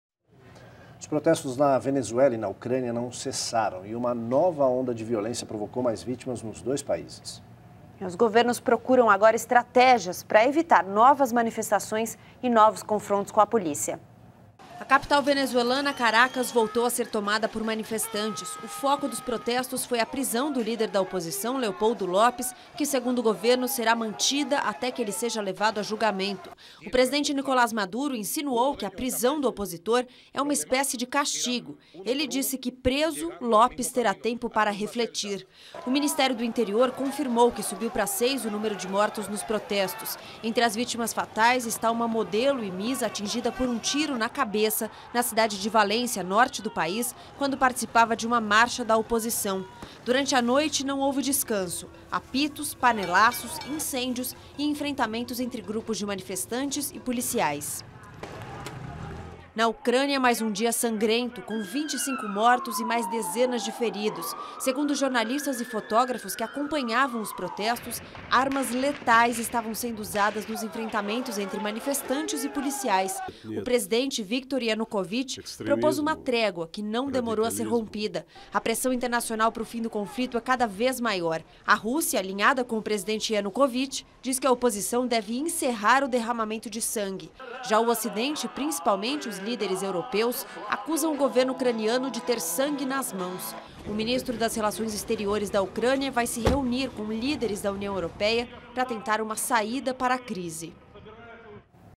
Onda de violência faz mais vítimas em protestos na Venezuela e Ucrânia
Os protestos na venezuela e na ucrânia não cessaram e uma nova onda de violência provocou mais vítimas nos dois países